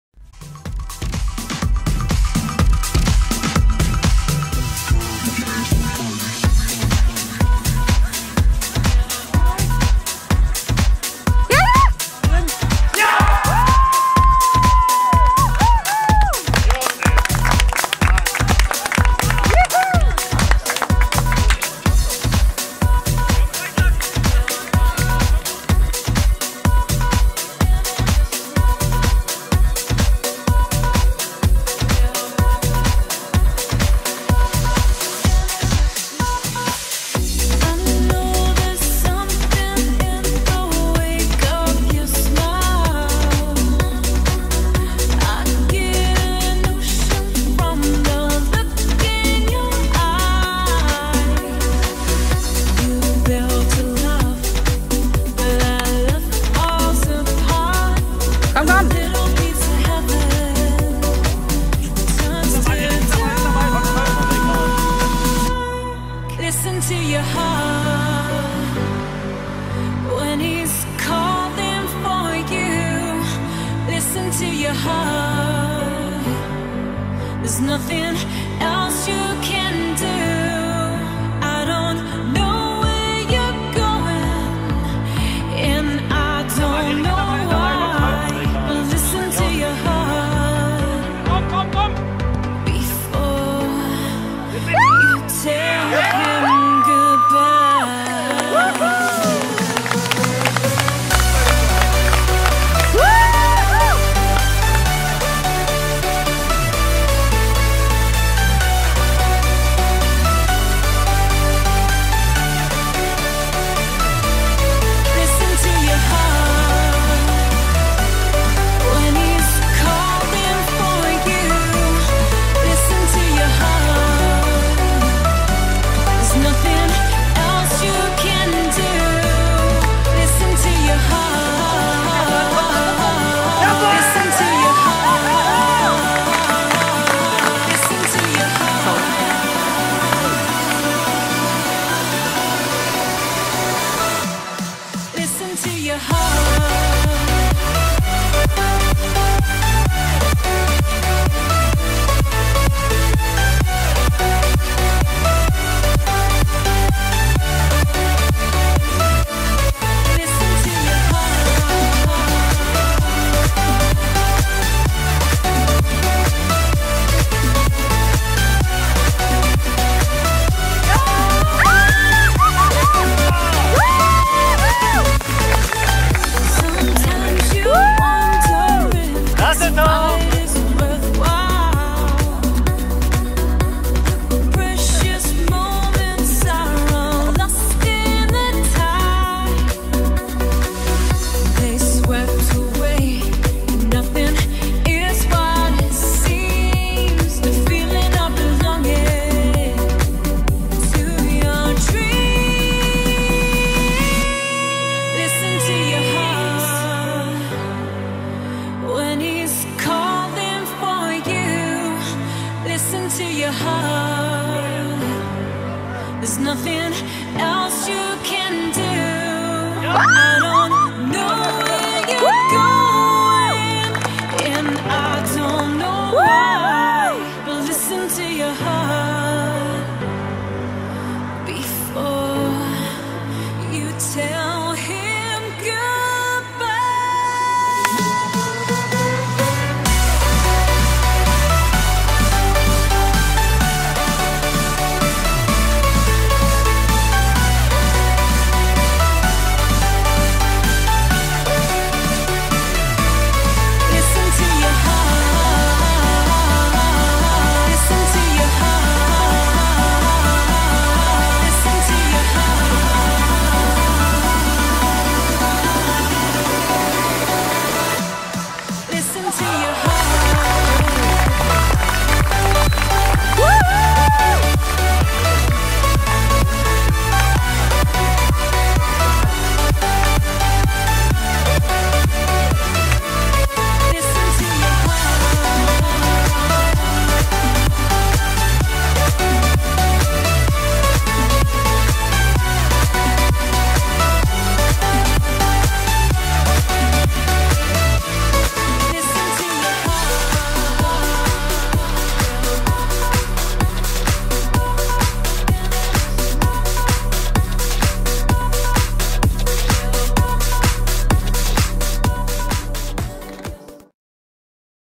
Tore MSV Zossen - Ludwigsfelder FC II
Hier die Tore der 1. Männer vom 7. Spieltag, als man 6:0 gegen Ludwigsfelde gewann!